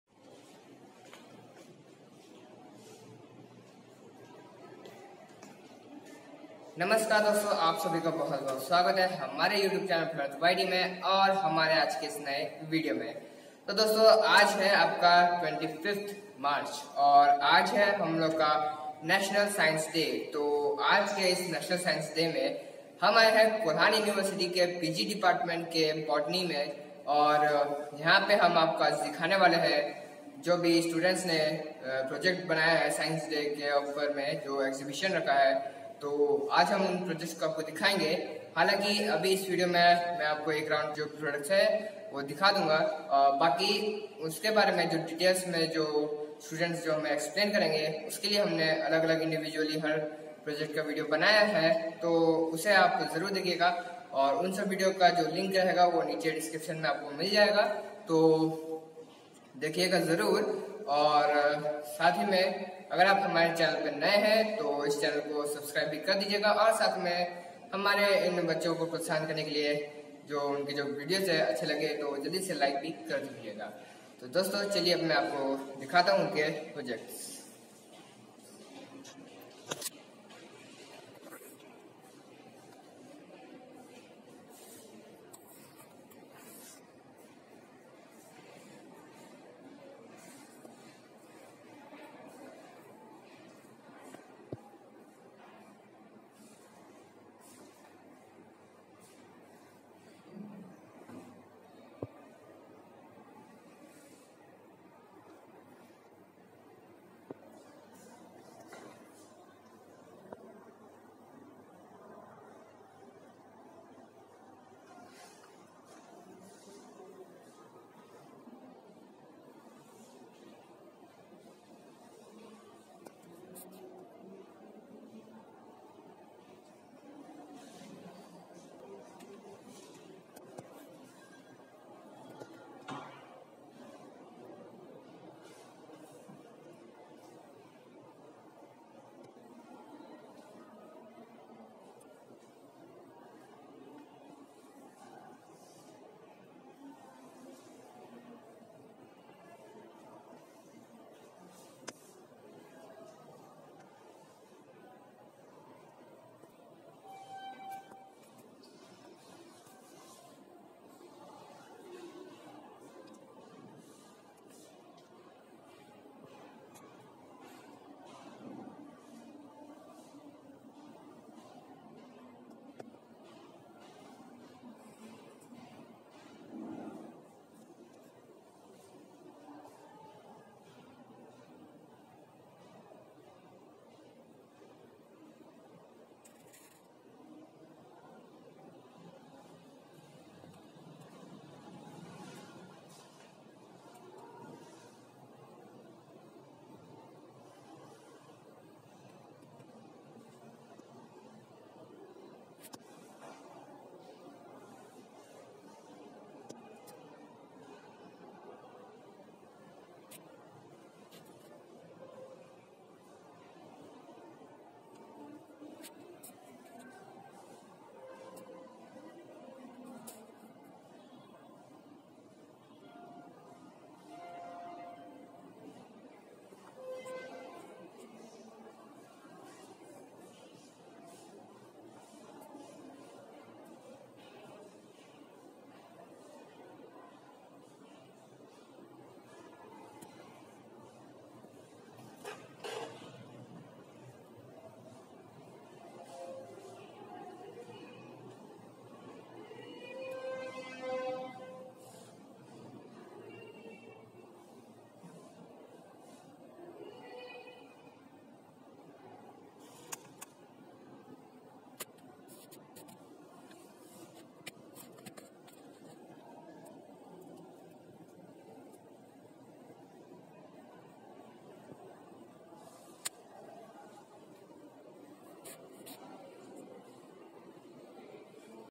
k.u botany science exhibition
namaskar dodto
to aj ka ish video ma ham ne batay hai ap ko national scince exhibition k bare  me juo ki serlebrst husa  tha kolhan university ka pg ka  botany depatment me